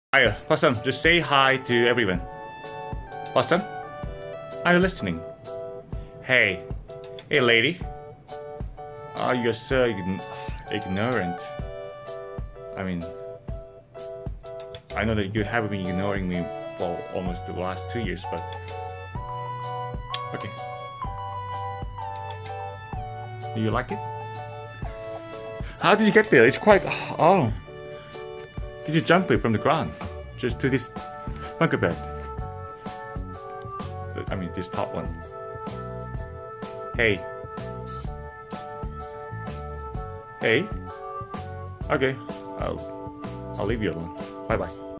Possum The ignorant
I tried to take some videos with her but she wouldn't react to me. More precisely, she just ignored as usual.